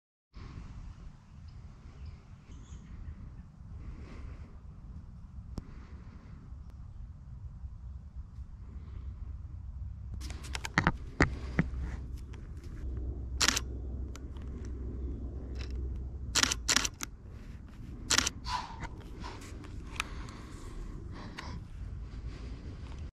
Wildlife Photography with Nikon D500 Nikon 200-500 filmed on GoPro Hero9
My go to cedar thicket for the November rut provides again.

Nikon D500
Nikon 200-500mm 5.6 on monopod
Quiet Continuous drive (Qc) mode
GoPro Hero9 on head mount